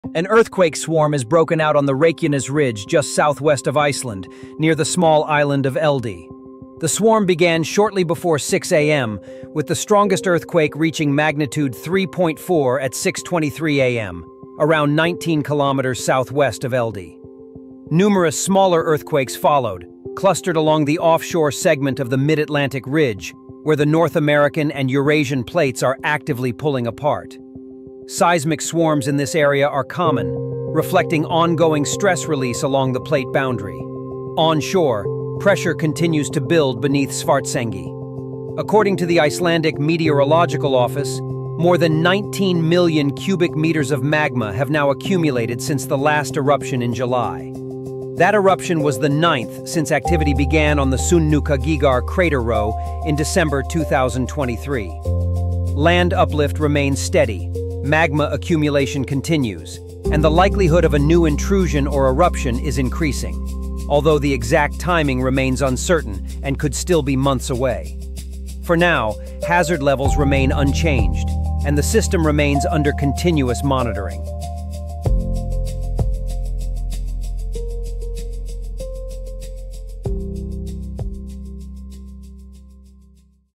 Earthquake Swarm Strikes Reykjanes Ridge
Earthquake swarm offshore near Eldey as magma continues to build beneath Svartsengi and the Sundhnúkagígar crater row. Ground uplift remains steady and pressure is still increasing across Iceland’s most active volcanic zone.